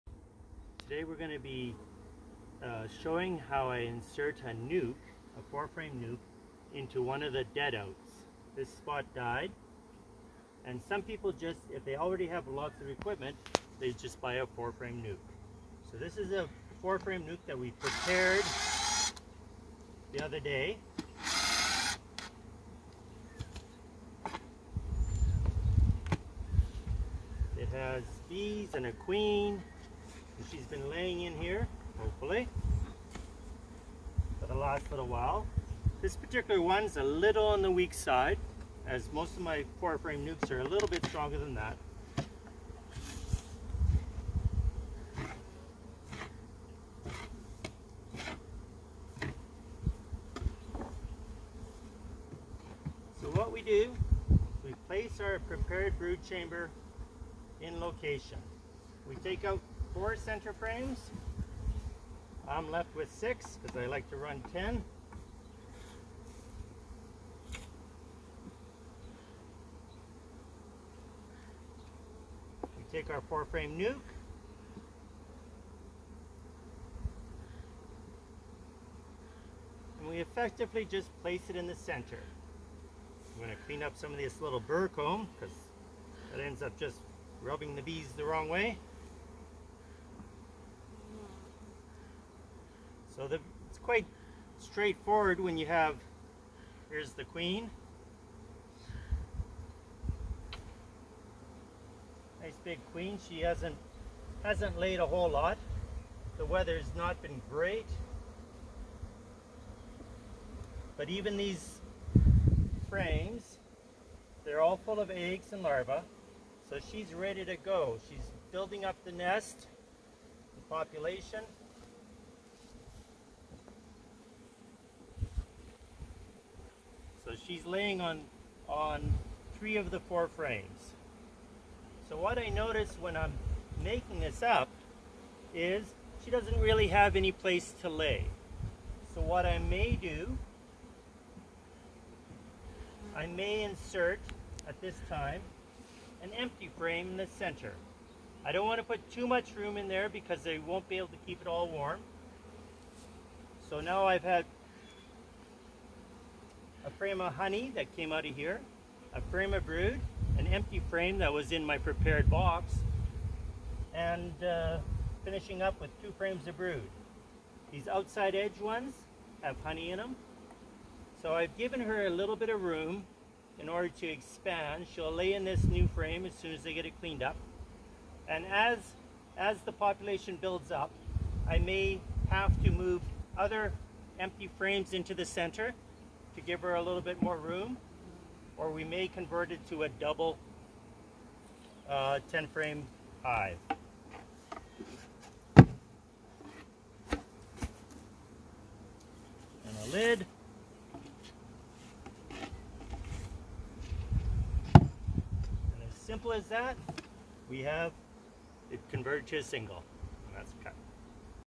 Hive blog #2 converting a nuc to a single
A quick look at converting a nuc to a single brood chamber hive.